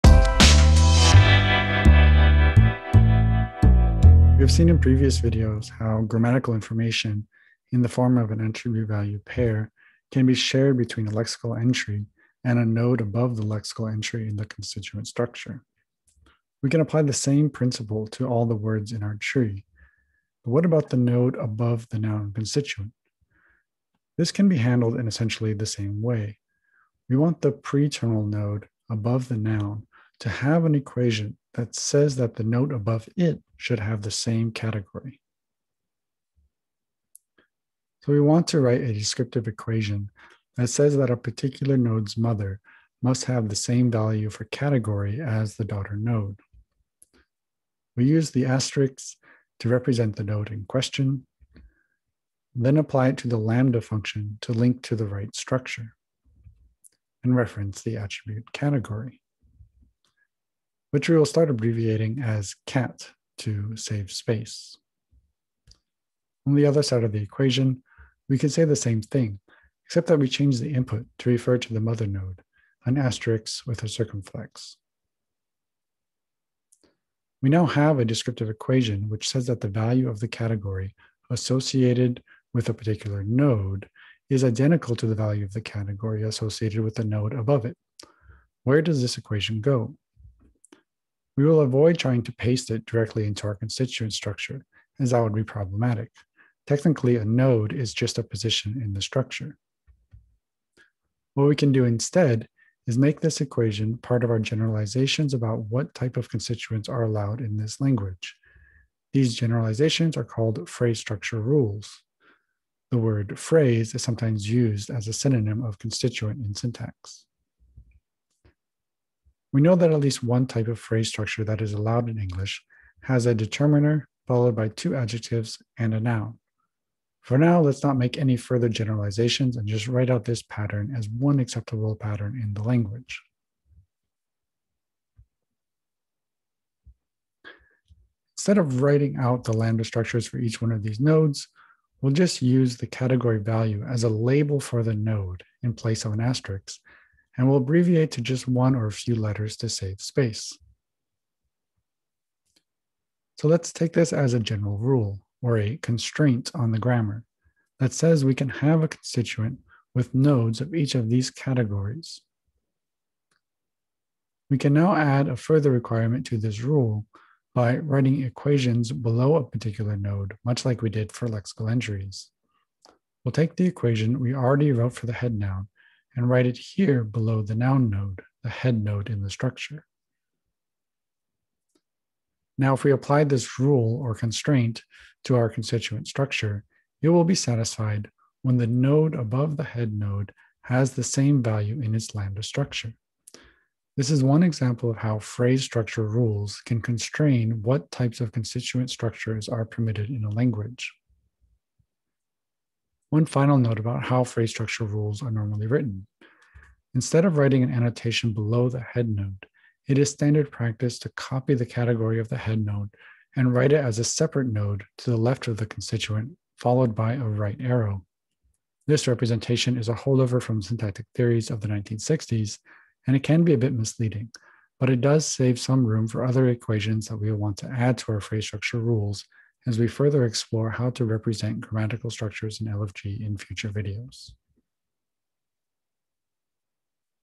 4 Lexical-Functional Grammar: A quick overview (video 4/10)
This video is part of an 11-video series giving a quick overview of Lexical-Functional grammar for those who already have some background in grammatical analysis.